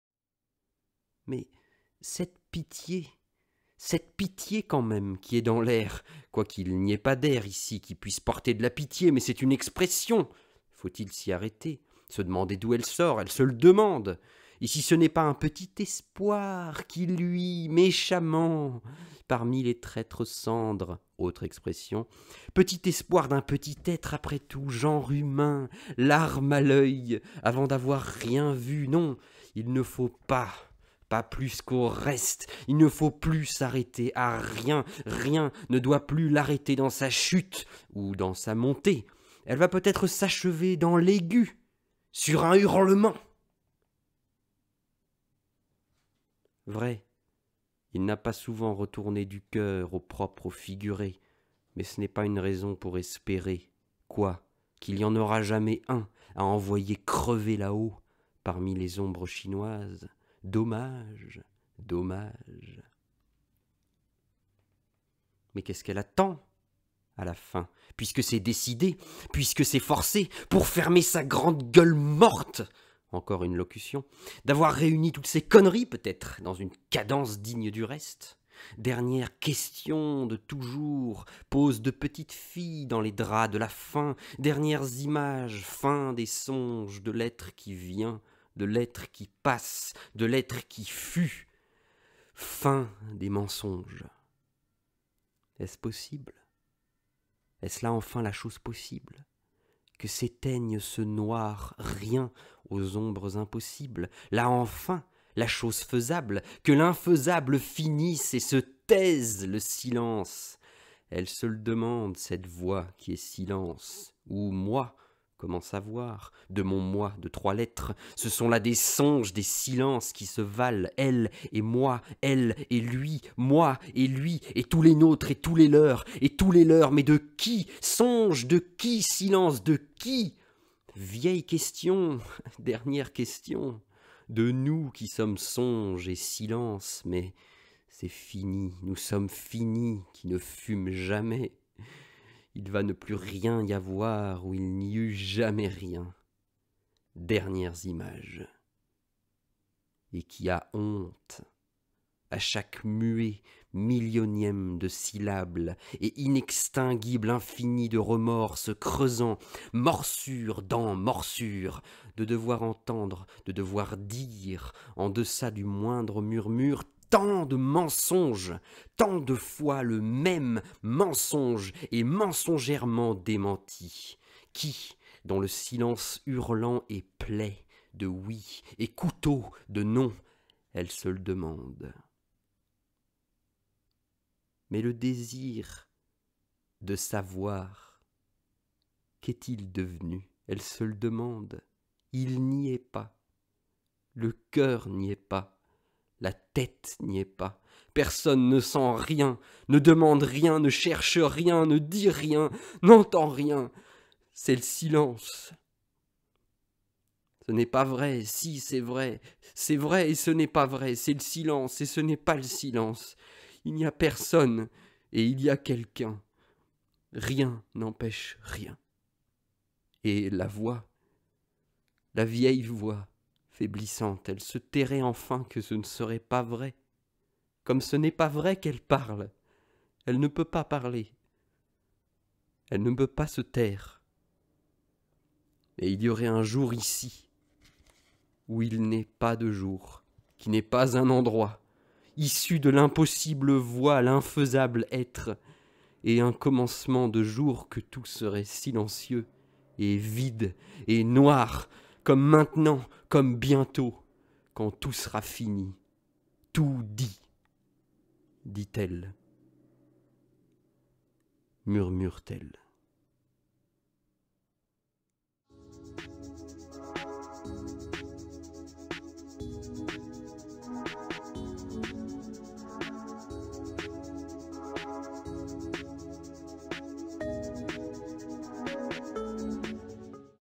Samuel Beckett : Nouvelles et textes pour rien (Chap.13) | Georges Z. Vernat
Samuel Beckett, Nouvelles et textes pour rien - Chap.13, extrait p. 203 à 206